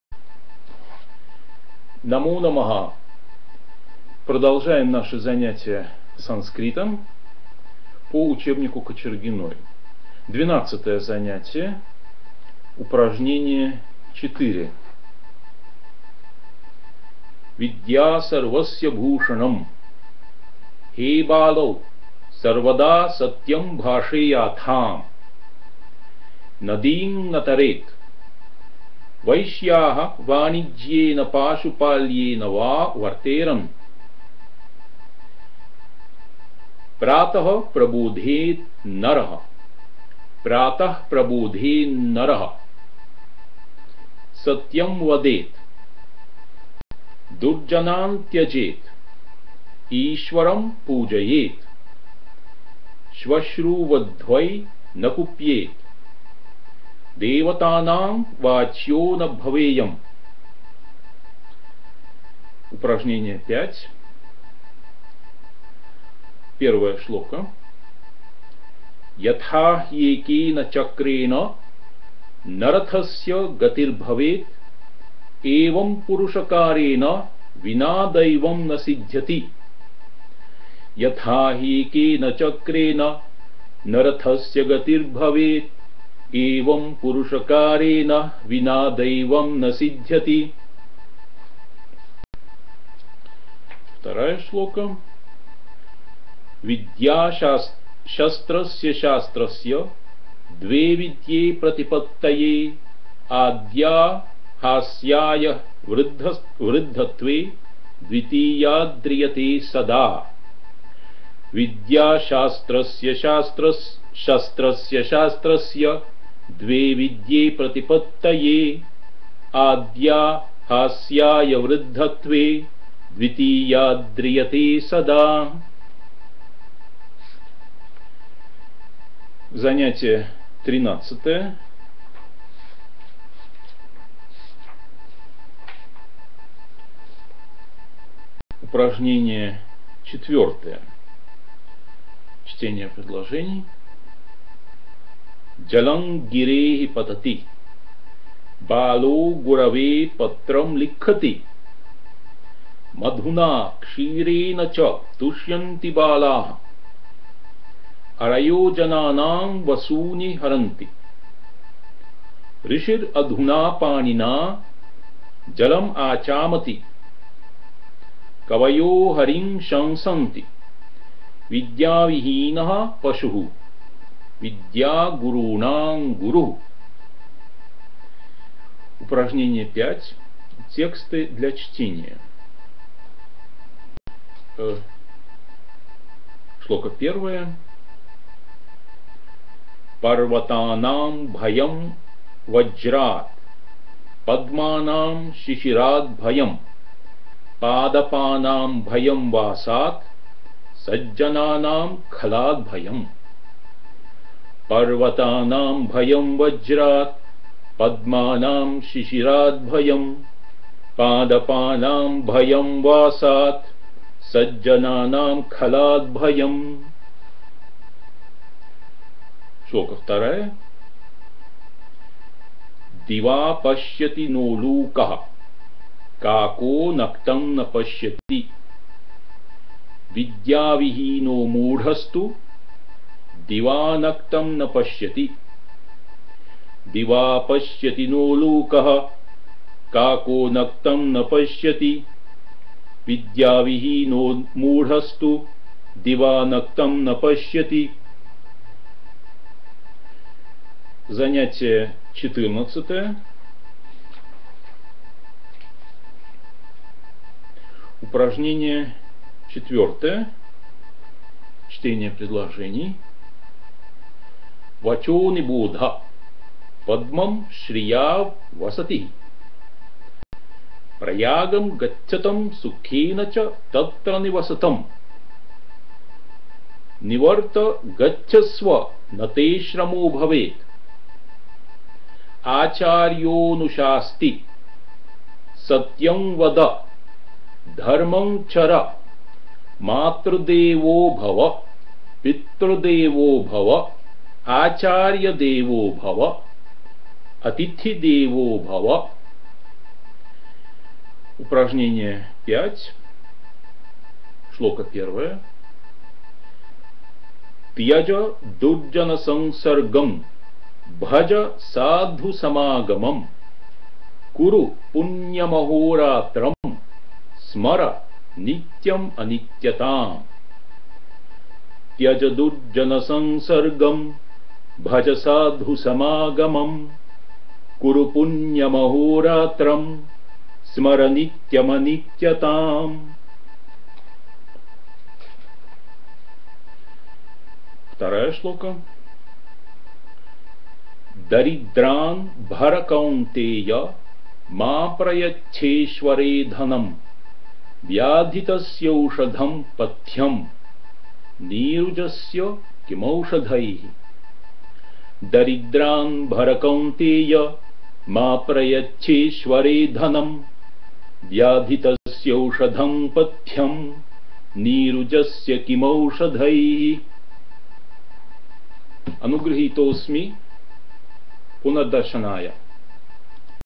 Учимся читать на санскрите (2)
Чтение вслух упражнений, написанных письмом деванагари, из "Учебника санскрита" В. А. Кочергиной (Москва: изд-во "Филология" 1994 г.): занятия XII -- XIV.
В помощь изучающим санскрит по этому учебнику.